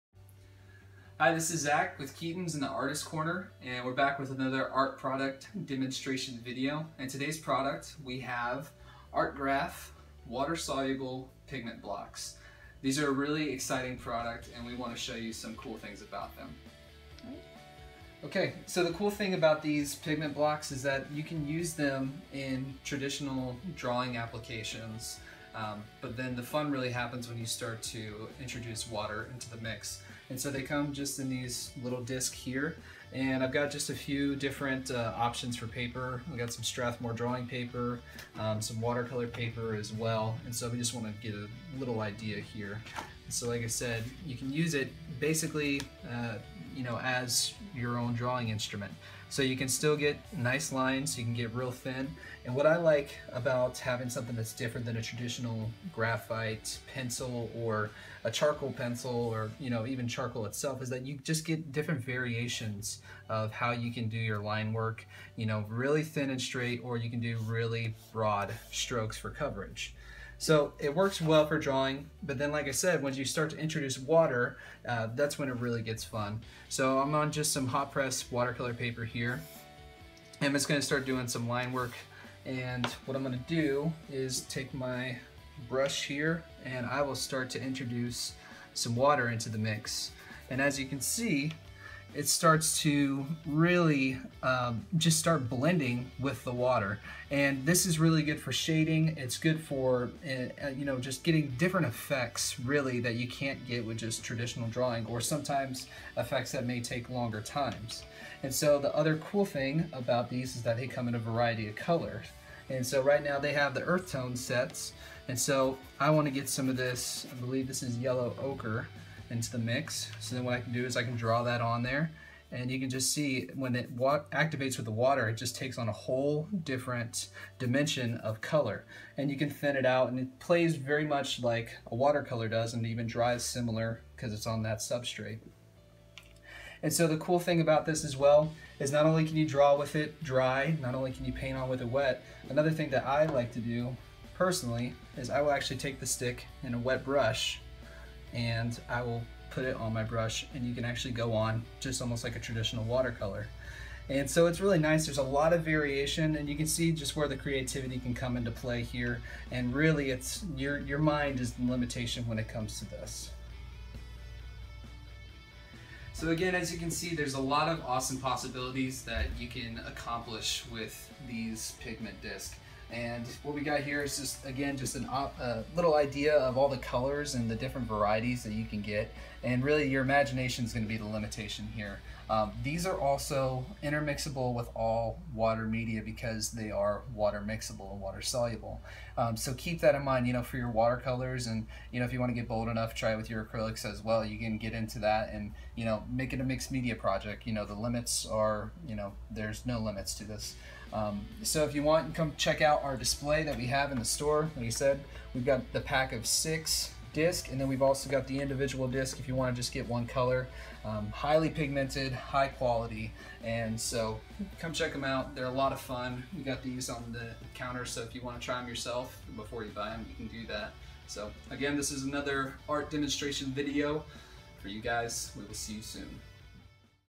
How to paint with graphite
Zac walks us through how to use ArtGraf's new product, water soluble colored graphite.